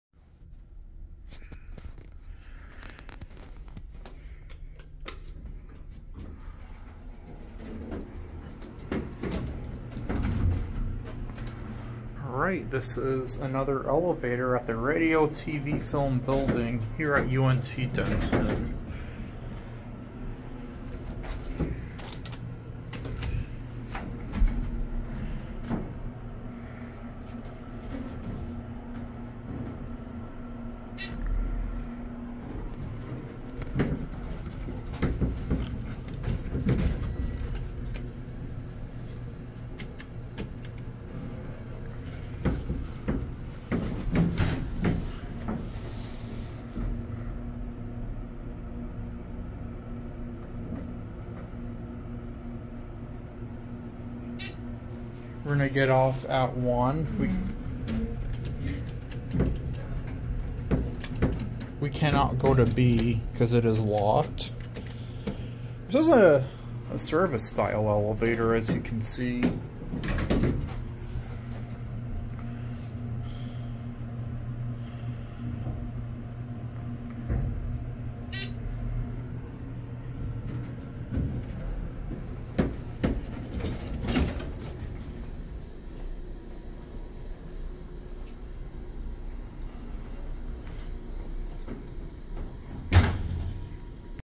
Dover Service Elevator Radio TV Film & Performing Arts University of North Texas (UNT) Denton, TX
Service Elevator Radio TV Film & Performing Arts The University of North Texas (UNT) Denton, TX

Brand: Dover
Speed: Hydraulic
Installed: 2000
Modernizations: None